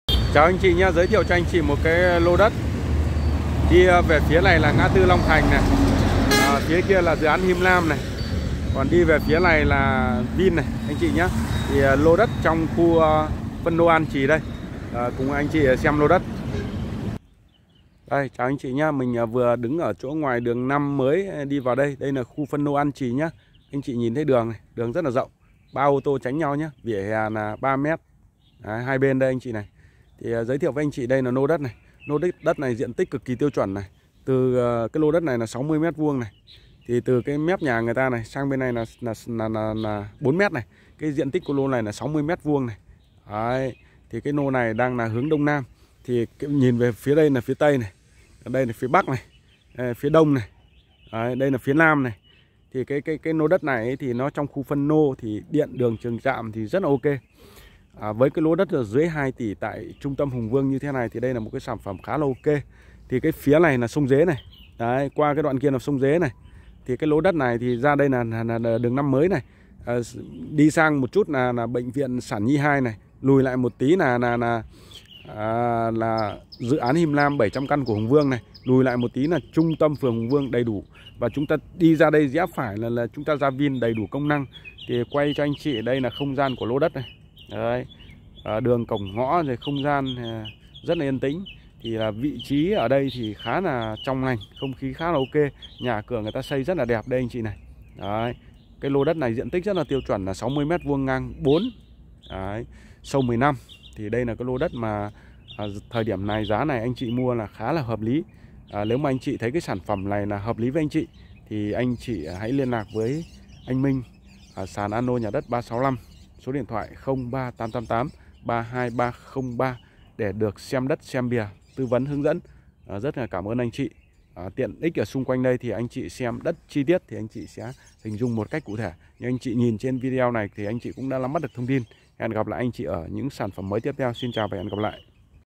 Bán đất Hồng Bàng, đường 5 mới, An Trì, Hùng Vương. Chỉ 1,85 tỷ, 60m2, đường 12m.0388.832.303
❤ Bán đất Hồng Bàng, đường 5 mới, An Trì, Hùng Vương. Chỉ 1,85 tỷ, 60m2, ngang 4m, đường 12m.0388.832.303
+ Đất phân lô, không lỗi lầm,  (4x15)m.
+ Đường trải nhưa 3 ô tô tránh nhau.
+ Vị trí ngay trung tâm phường Hùng Vương, gần trường cấp 1, cấp 2, mầm non, Ủy Ban,
+ Gần BV Giao Thông, Ô tô For, ngay ngã tư đèn xanh Đỏ.
+ Cạnh dự an Him Lam Hùng Vương.
+ Sản phẩm đầu tư sinh lời.
+ Sổ đỏ chí chủ (xem đất xem sổ).
❤ Giá chỉ 1,85 tỷ.
+ Gọi ngay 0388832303 Anh Minh (24/24).
+ Cam kết thông tin, hình ảnh thật 100%.
+ Xem thêm
+ Sàn BĐS
+ Tp01.30.31.DB